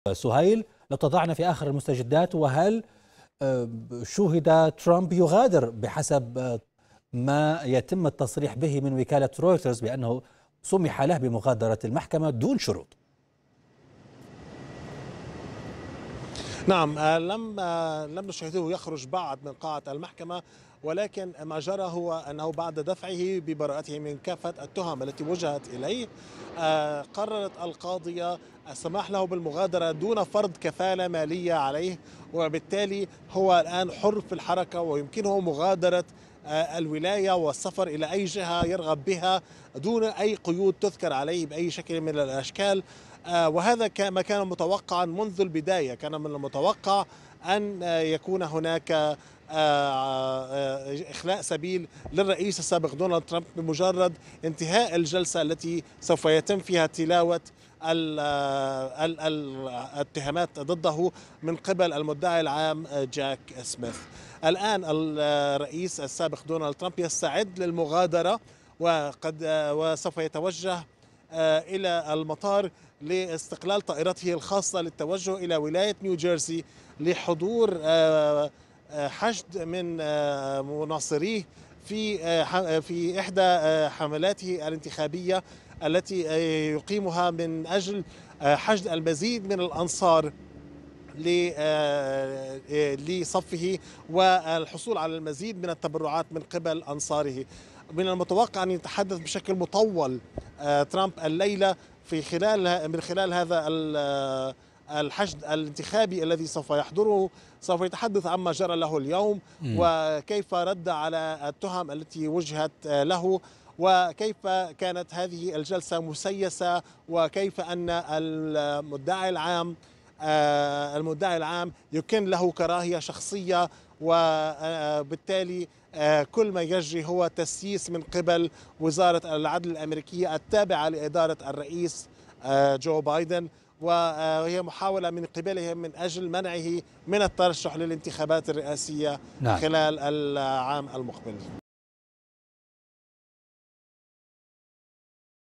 مراسلنا: السماح لترمب بمغادرة المحكمة دون فرض كفالة
دفع الرئيس الأميركي السابق دونالد ترمب ببراءته من جميع التهم الموجهة إليه وقال إنه غير مذنب، جاء ذلك خلال مثوله أمام محكمة ميامي الفيدرالية في قضية احتفاظه بوثائق سرية تعرض أمن الولايات المتحدة للخطر.
وكان ترامب قال في تصريح مقتضب له على منصته "تروث سوشال" إن هذا اليوم أكثر الأيام حزنا في البلاد، مكررا اتهاماته بأن المحاكمة ملاحقة سياسية له. هذا ونقلت وكالة رويترز عن وسائل إعلام أمريكية أنه سمح لترامب بمغادرة المحكمة دون شروط.. مزيد من التفاصيل يتحدث من ميامي بولاية فلوريدا مراسل الغد سهيل الشاعر مع الإعلامي نبيل الحاج.